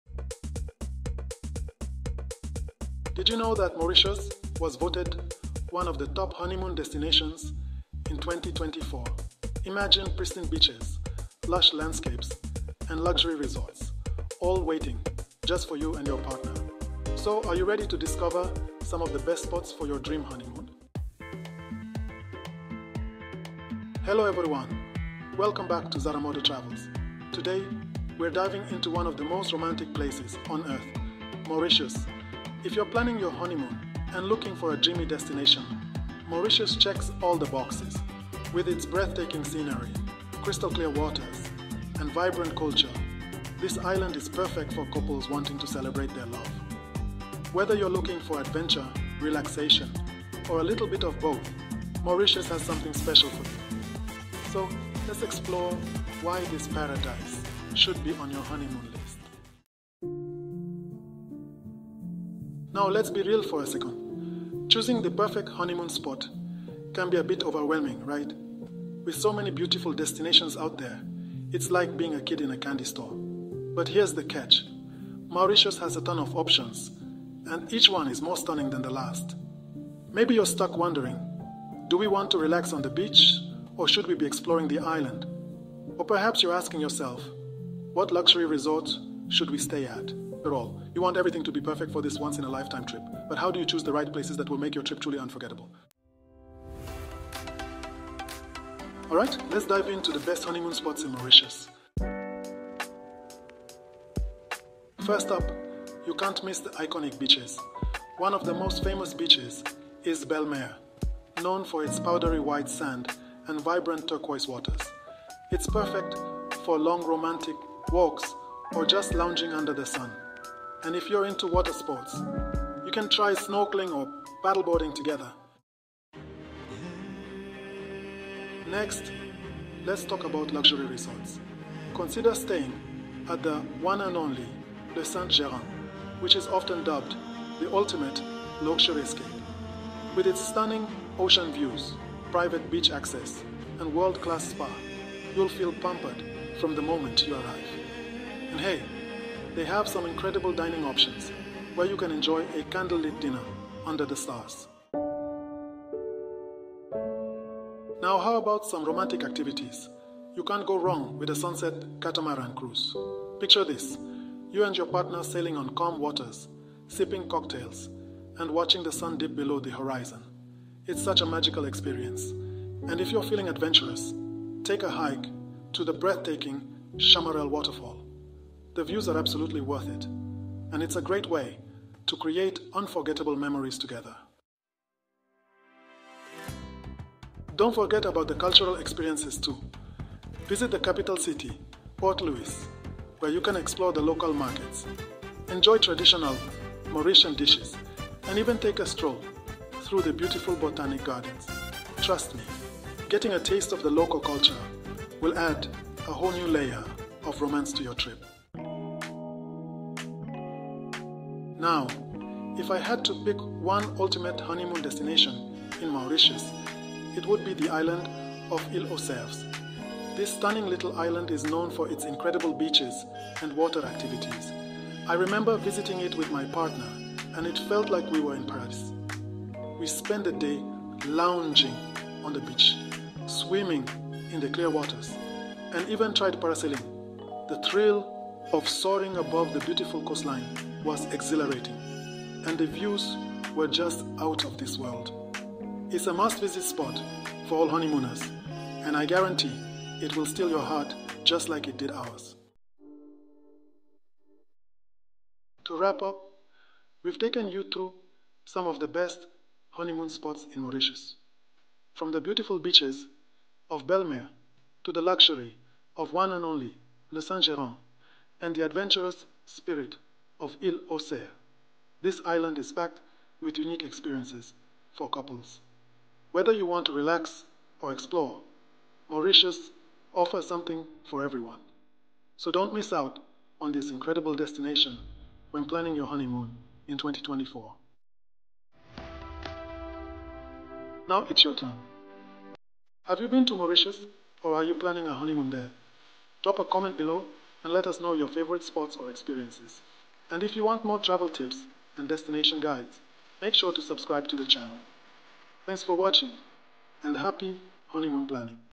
Best Mauritius Honeymoon Spots You HAVE to Visit
In this video, we explore the idyllic beaches, luxurious resorts, and enchanting landscapes that make Mauritius a top destination for newlyweds. From the stunning shores of Belle Mare to the vibrant nightlife of Grand Baie, we’ll guide you through the best places to create unforgettable memories and the paradise cove mauritius honeymoon.

Join us as we share insider tips on where to stay, what to do, and how to make the most of your honeymoon in this tropical paradise. Whether you’re looking for relaxation, adventure, or a mix of both, Mauritius has something for every couple. Don’t miss our recommendations for romantic dining experiences and exciting excursions that will make your mauritius island honeymoon truly special.

Make sure to watch until the end for the ultimate travel tips to help you plan your dream honeymoon in Mauritius!